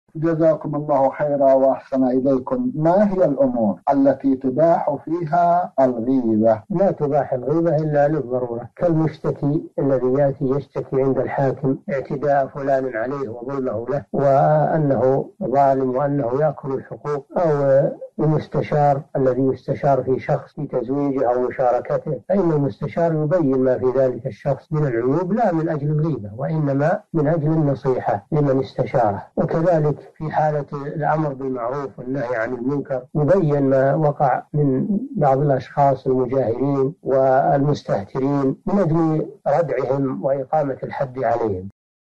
ما هي الأمور التي تباح فيها الغيبة؟ الشيخ صالح بن فوزان الفوزان
نص السؤال: نص السؤال: ما هي الأمور التي تُباح فيها الغيبة؟
السيرة الذاتية :
للاشتراك في قناة الشيخ صالح الفوزان

رئيس الفريق
لمشروع كبار العلماء